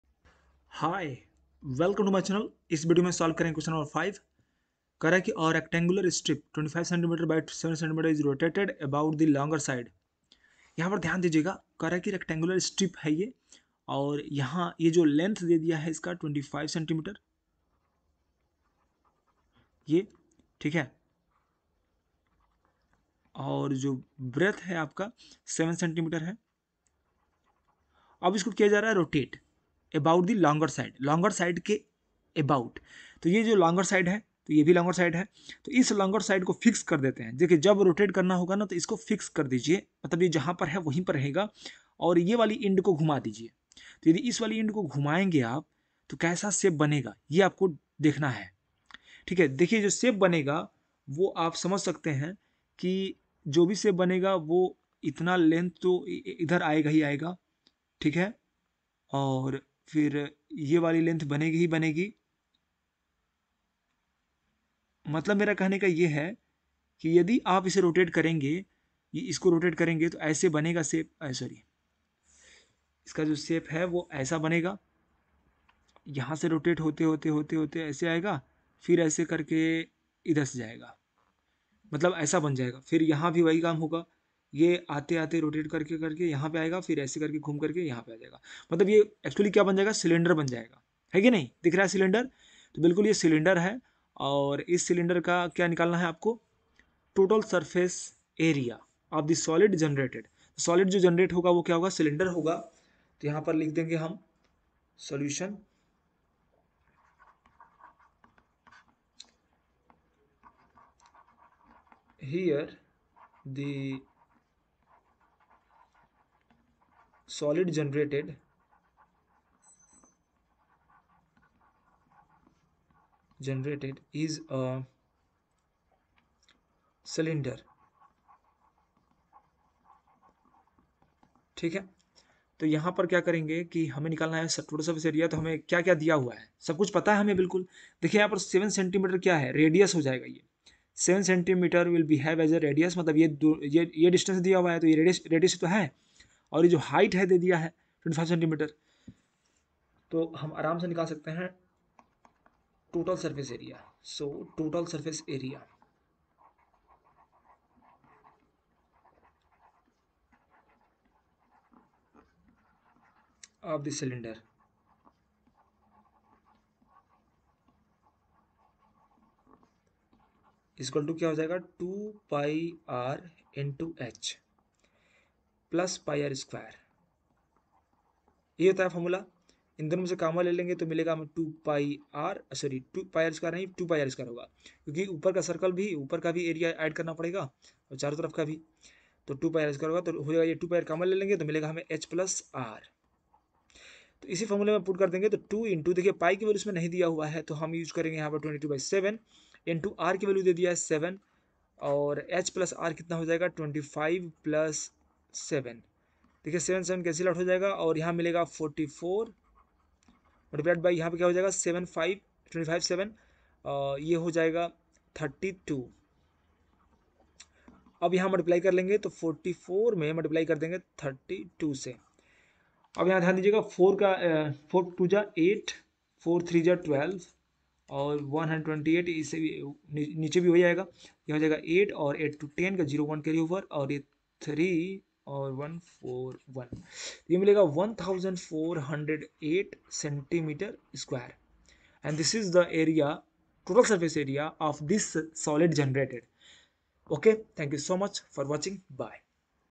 Q.5 - Ex 22.1 Chapter 22 Volume and Surface Area of a Cylinder R D Sharma Class 8 Math Solutions
5. A rectangular strip 25 cm by 7 cm is rotated about the longer side. Find the total surface area of the solid thus generated.
Question no. 5 Exercise 22.1 Chapter no. 22 Mensuration III Volume and Surface area of a right circular cylinder R D Sharma Class 8 Math solutions